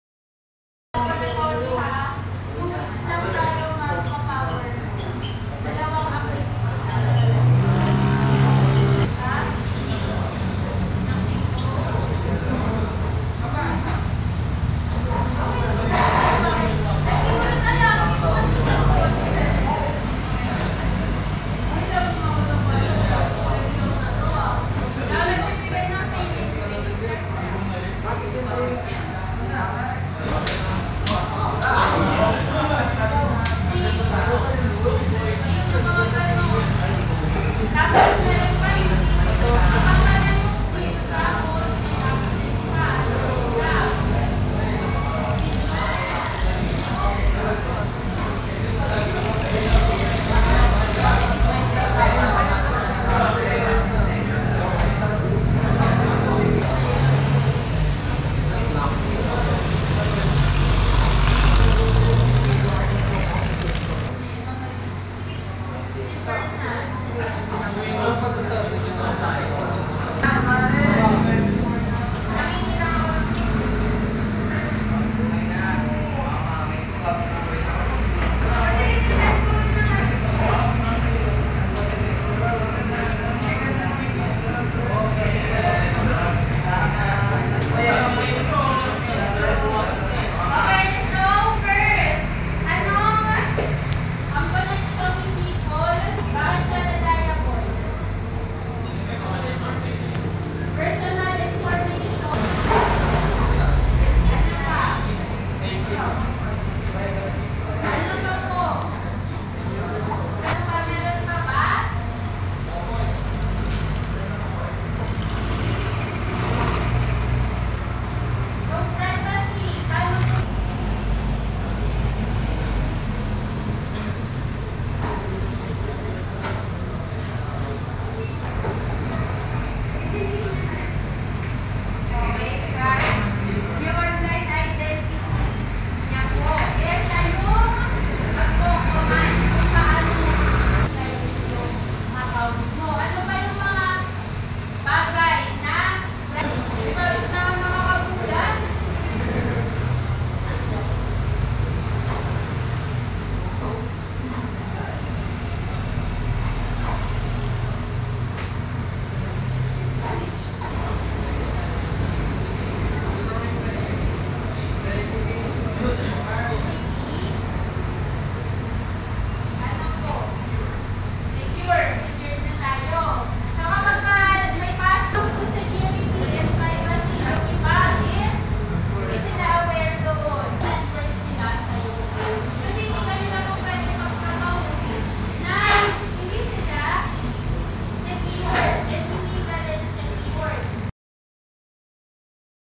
Philippine Buses, Mini Buses & Jeepneys At Cavsu Dalahican Cavite City Part 2 of 2
Bus Spotting, Mini Bus Spotting & Jeepney Spotting At Room 217 Of Cavsu Dalahican Cavite City Part 2 Finale